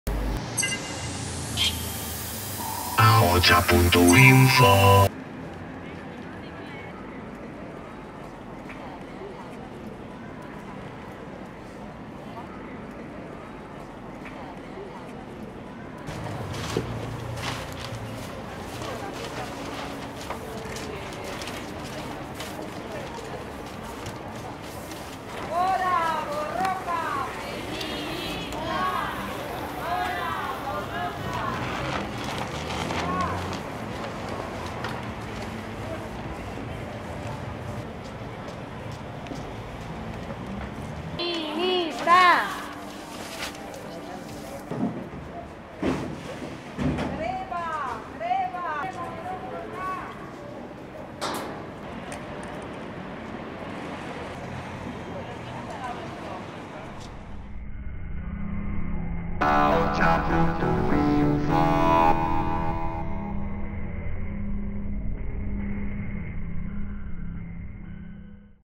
U30GrebaOrokorra Organizan una acción feminista 12 días antes de la huelga general
♀ Por un trabajo, una vida y unas pensiones dignas, el colectivo feminista organiza una ekintza para calentar motores, en una conocida multinacional de ropa en Iruñea.